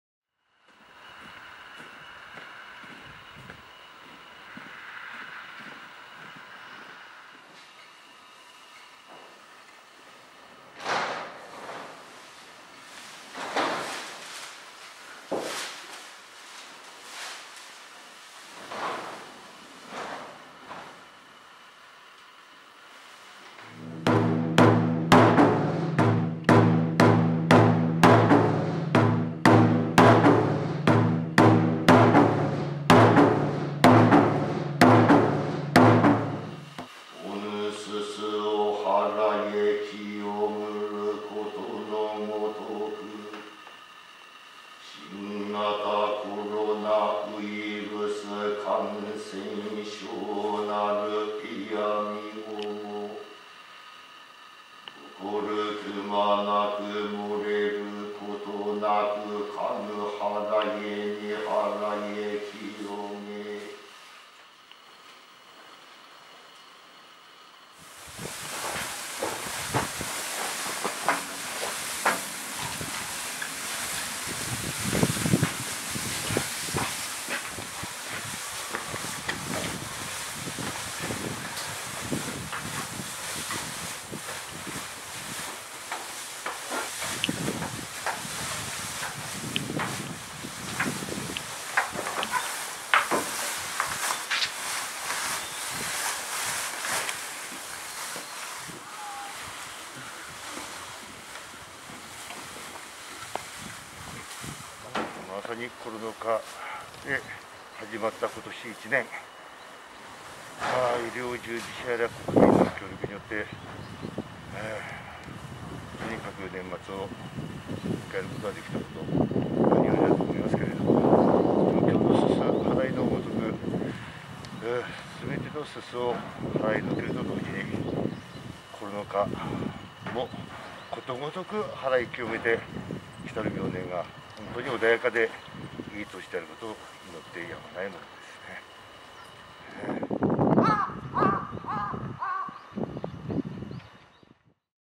すす払い 丁寧に／平川・猿賀神社
青森県平川市の猿賀神社は22日、年末恒例の「煤払祭（すすはらいさい）」を行った。神職ら5人が1年間で本殿や拝殿にたまったほこりを払って建物を清め、新年を迎える準備を整えた。